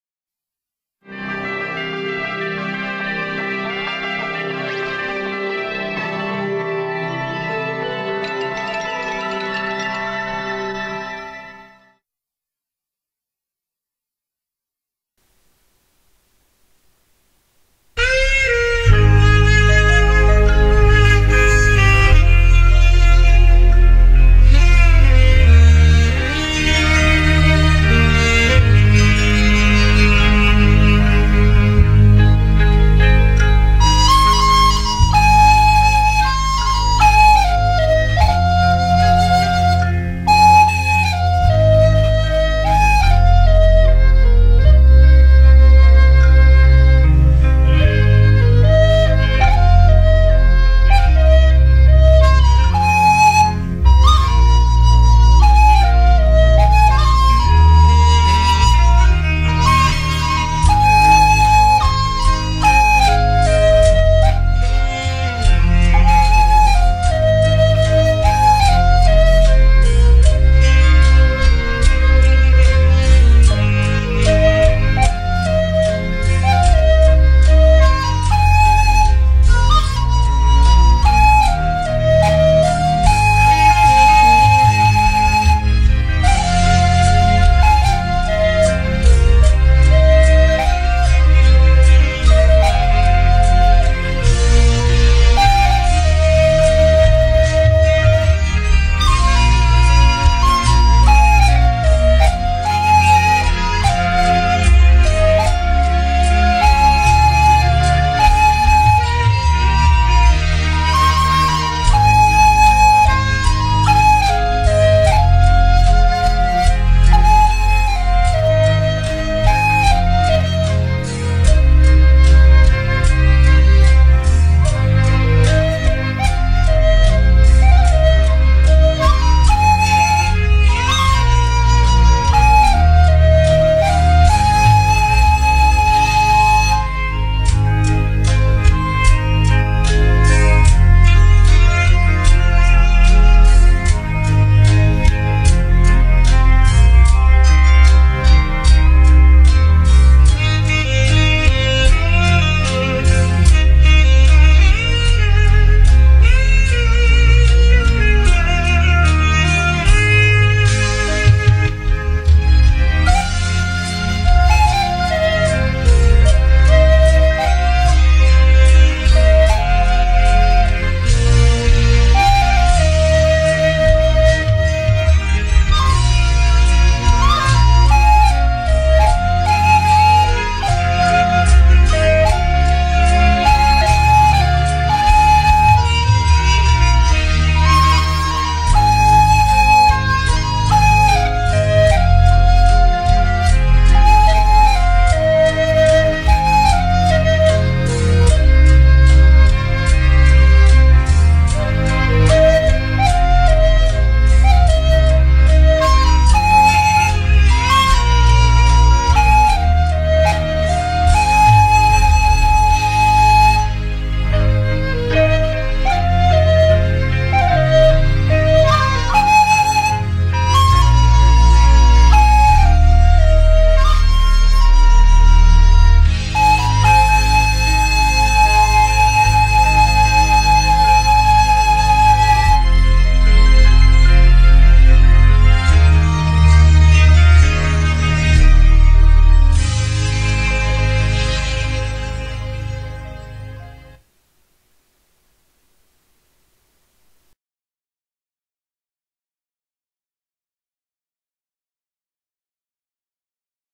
今天的我 笛子試吹(用亚美利坚进口环保可分解垃圾袋代替芦苇笛膜)。
用亚美利坚进口环保可分解垃圾袋代替芦苇笛膜。